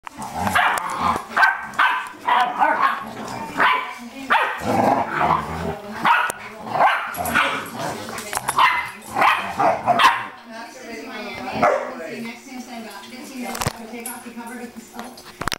Pit Bull Puppies
8 weeks old, playing & barking with each other & their parents!